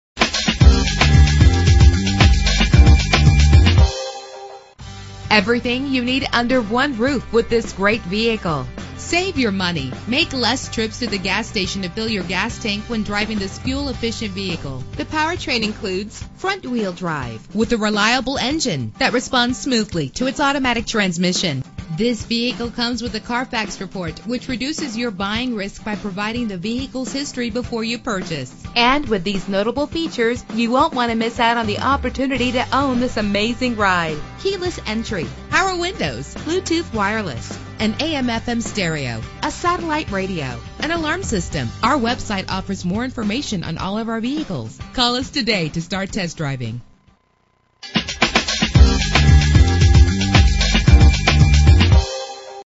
2013 Chevrolet Cruze - Wind Gap PA
Year: 2013
Make: Chevrolet
Model: Cruze
Trim: LS
Engine: 1.8 L, 4 cylinder
Transmission:
Color: White
Mileage: 23171
Address:
1043 South Broadway